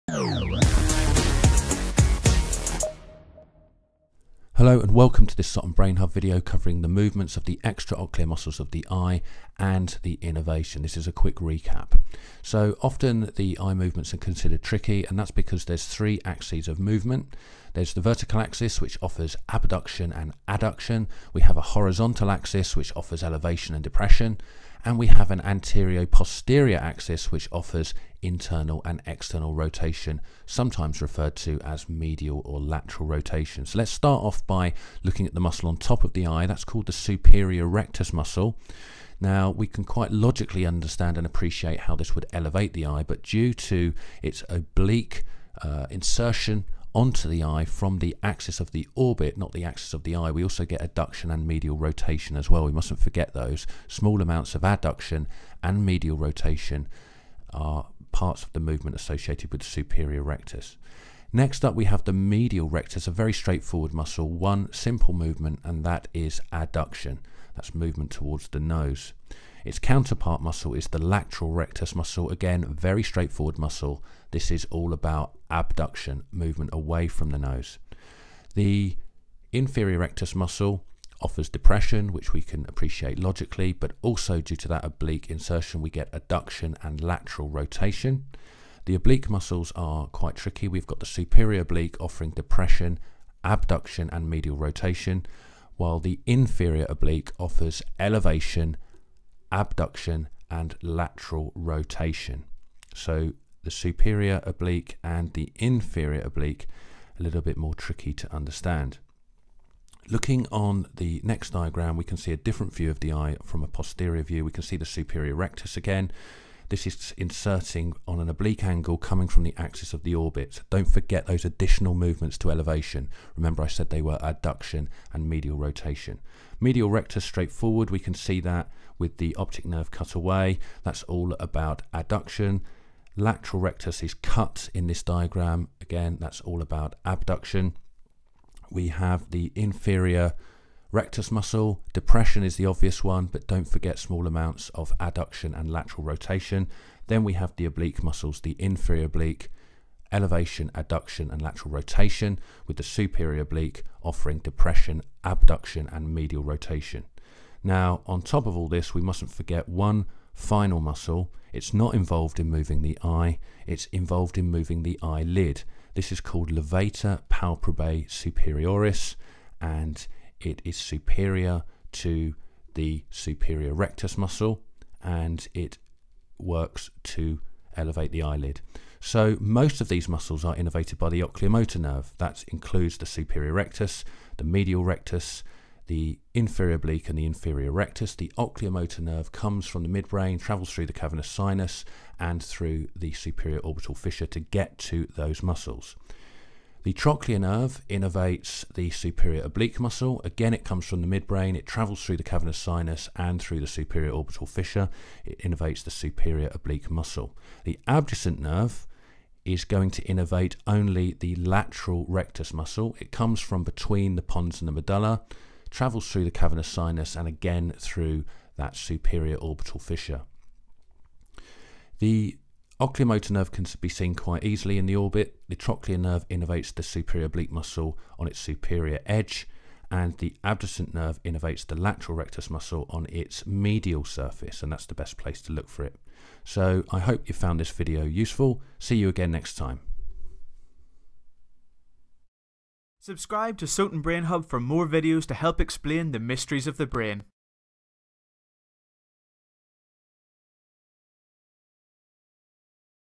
Extraocular Muscles of The Eye and their Innervation
In this video Scott talks us all through the muscles which control the movements of the eyes along with the cranial nerves which innervate them.